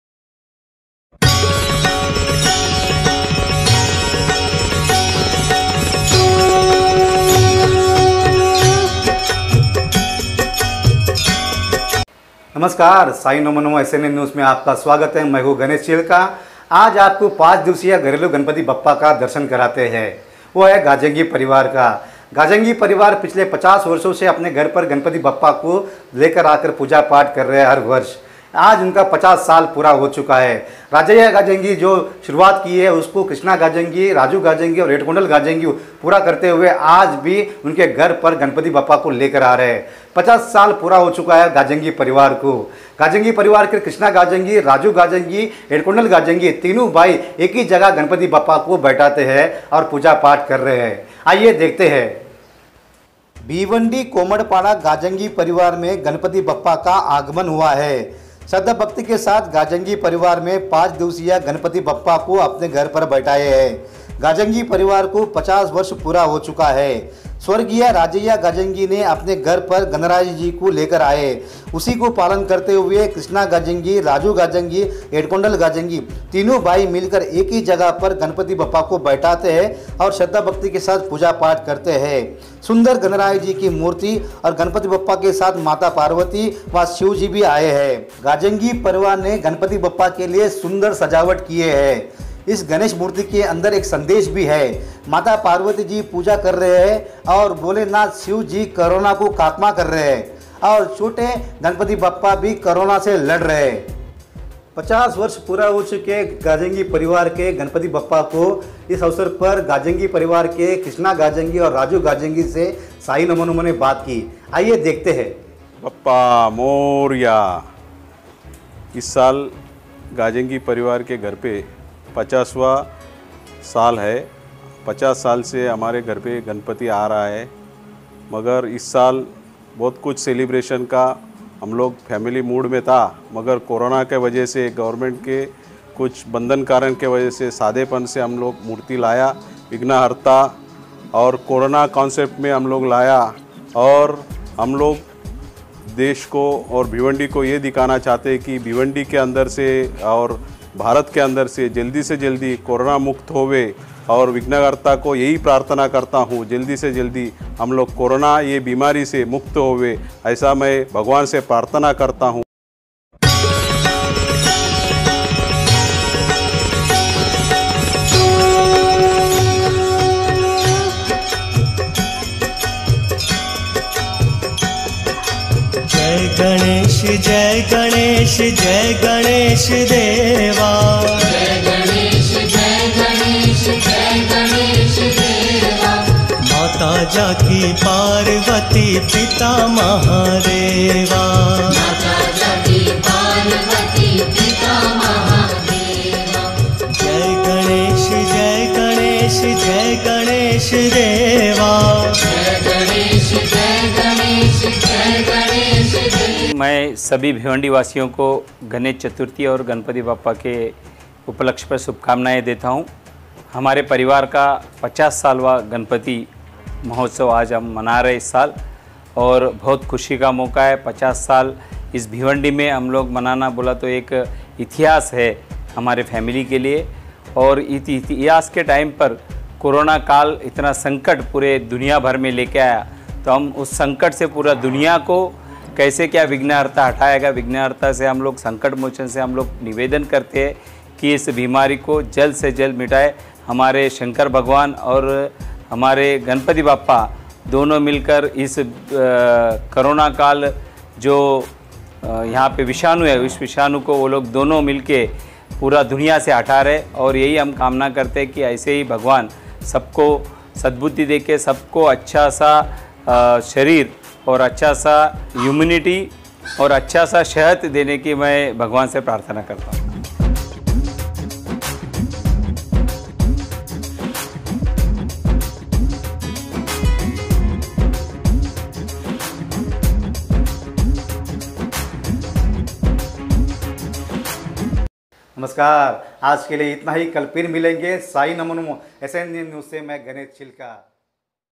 The 50th Ganeshotsav of the Gajengi family is being performed with devotion at his home. SNN, News.
Songs - GaneshjikiAarti.Com
Album - GaneshjikiAarti.Com
Signer - GaneshjikiAarti.Com

The Song's Played during Telecast are the Exclusive Property of Respected Music Company.

The 50th Ganeshotsav of the Gajengi family is being performed with devotion at his home.
SNN, News. Editor - Ganesh Chilka